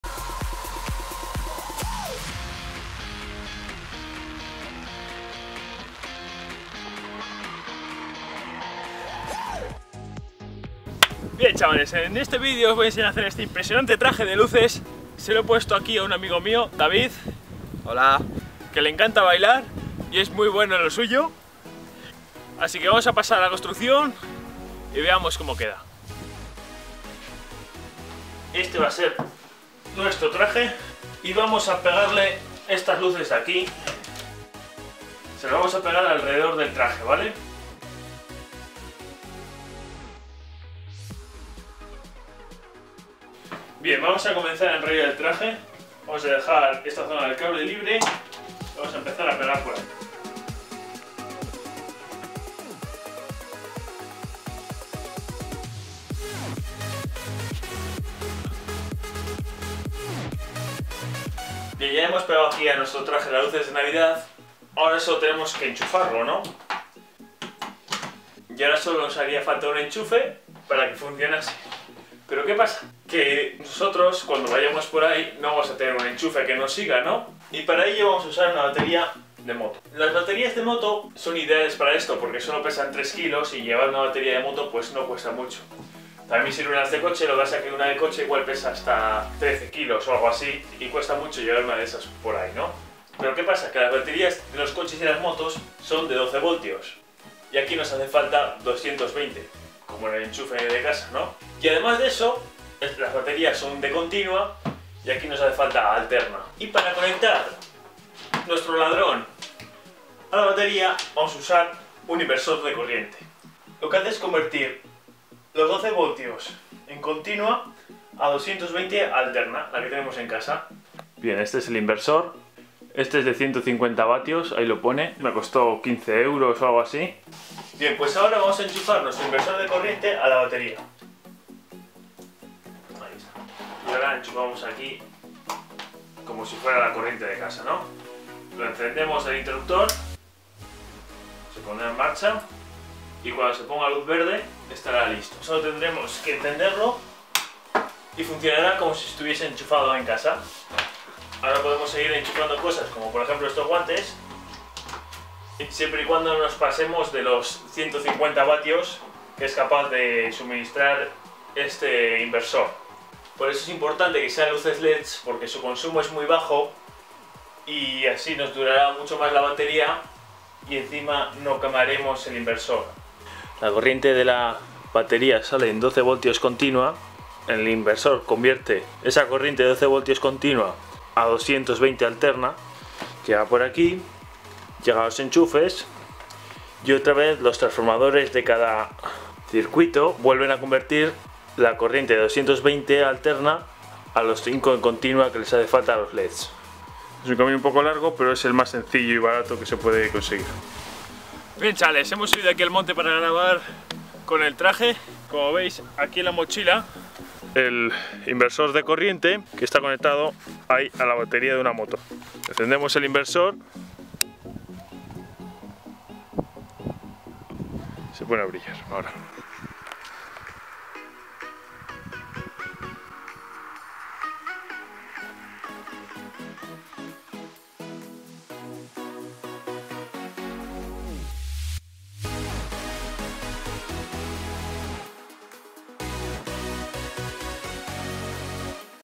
Como Fabricar un TRAJE DE LUCES
En este vídeo os enseñaré a fabricar este impresionante traje de luces de navidad con una batería de moto y un inversor de corriente.